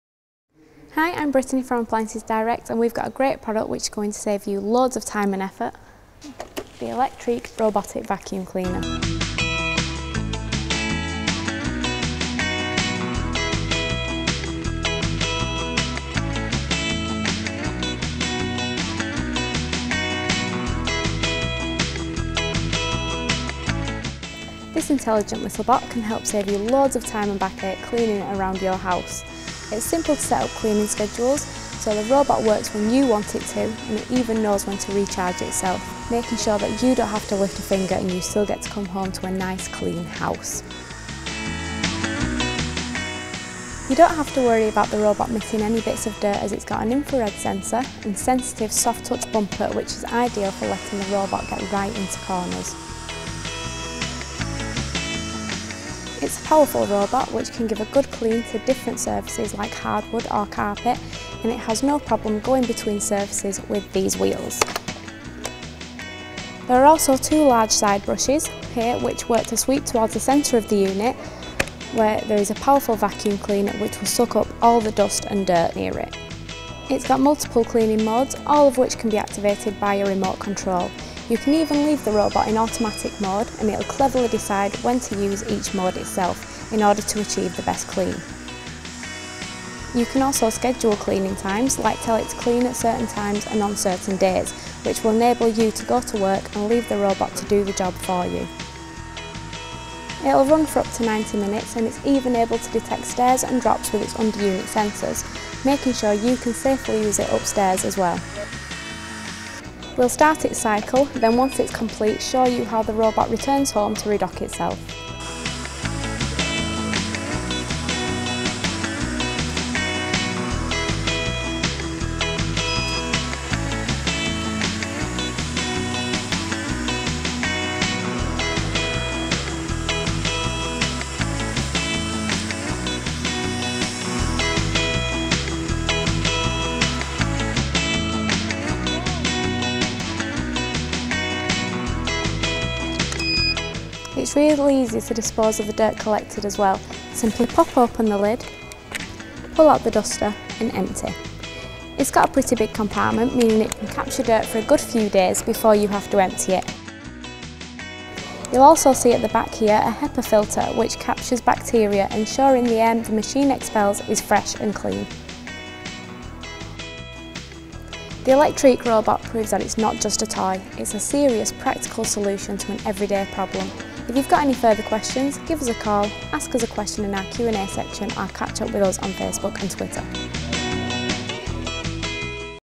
ElectrIQ Robotic KK8 Vacuum Cleaner
Here's a superb item that's going to save you bags of time and effort. The KK8 Robotic Vacuum Cleaner!

Tired of cleaning up after the kids everyday? Tiring of picking up pet hairs and dirt all the time? Then this is perfect solution for you. The robotic vacuum can be scheduled to clean you house every day and keep everything in your home clean and tidy, without you lifting a finger!